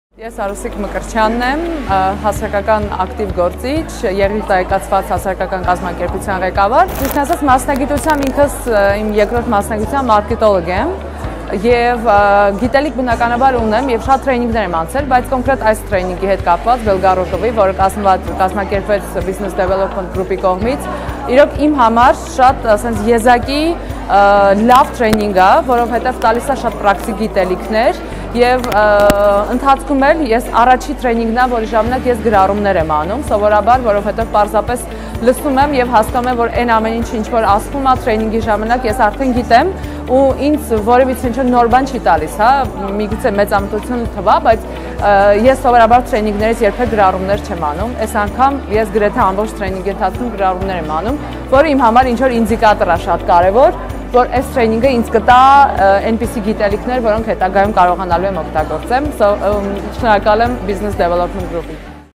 Reviews | Flight to Marketing | Արուսիկ Մկրտչյան
Շնորհակալ ենք Արուսիկ Մկրտչյանին «𝗙𝗹𝗶𝗴𝗵𝘁 𝘁𝗼 𝗺𝗮𝗿𝗸𝗲𝘁𝗶𝗻𝗴» թրեյնինգից ստացած տպավորությունների մասին պատմելու համար 🤗

Ձեր կարծիքները մոտիվացնում են մեզ կազմակերպել ավելի օգտակար և մաշտաբային թրեյնինգներ 😍